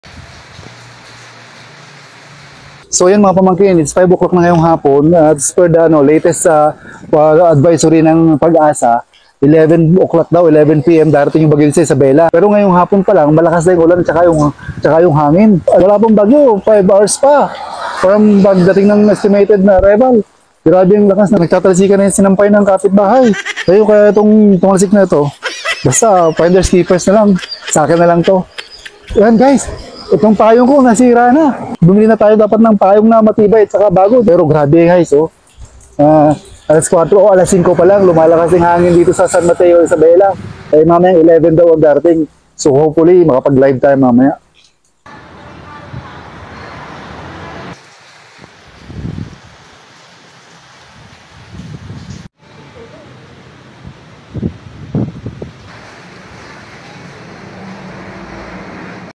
Hagupit ni Kristine, ramdam na agad kahit 5 hours pa bago tumama dito sa Isabela!
Ngayong hapon pa lang ramdam na ang lakas ni Kristine dito sa Isabela.